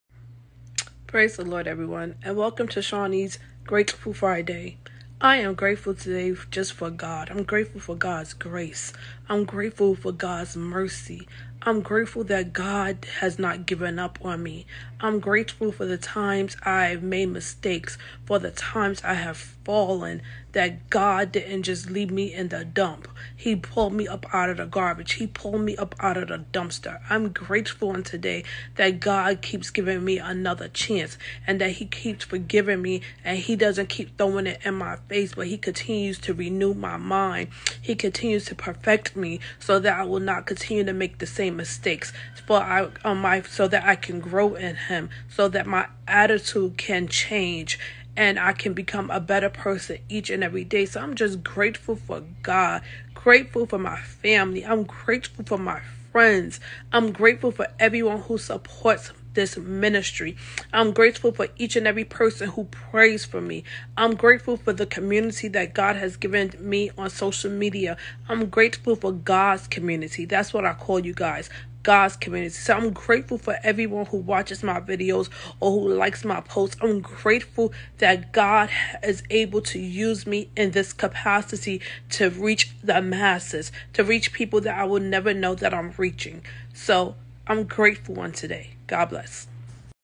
Shawnee's Grateful Friday
“Shawnee’s Grateful Friday: I Am Grateful For Our Almighty Father! I’m Grateful That He Pulled Me Up When I Was Down. I’m Grateful That He Doesn’t Throw Me Away When I Fall Or Mess Up. I’m Grateful That He’s A Compassionate, Loving And Forgiving Father. I’m Grateful For His Grace And Renewing Mercy. I’m Truly Grateful For You! Thank You For All Of Your Support, I’m Grateful!”